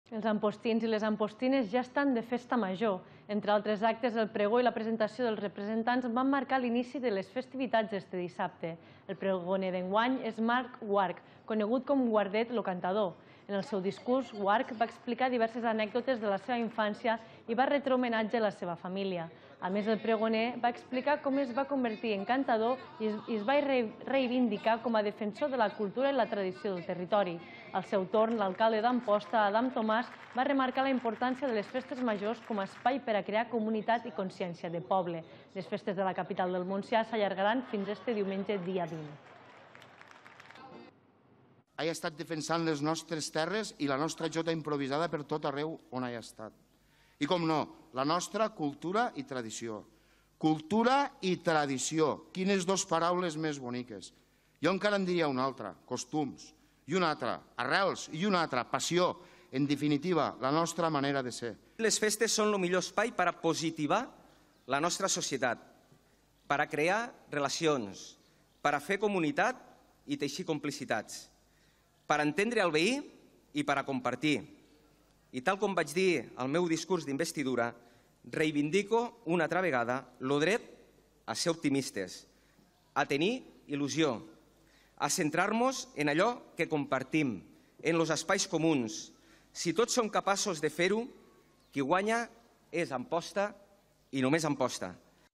El pregoner d'Amposta Guardet lo cantador reivindica la cultura i la tradició del territori
Els ampostins i les ampostines ja estan de festa major. Entre altres actes, el pregó i la presentació dels representants van marcar l’inici de les festivitats este dissabte. El pregoner d’enguany és Marc Guarch, conegut com a Guardet lo cantador. En el seu discurs, Guarch va explicar diverses anècdotes de la seva infància i va retre homenatge a la seva família. A més, el pregoner va explicar com es va convertir en cantador i es va reivindicar com a defensor de la cultura i la tradició del territori. Al seu torn, l’alcalde d’Amposta, Adam Tomàs, va remarcar la importància de les festes majors com a espai per crear comunitat i consciència de poble. Les festes de la capital del Montsià s’allargaran fins este diumenge dia 20.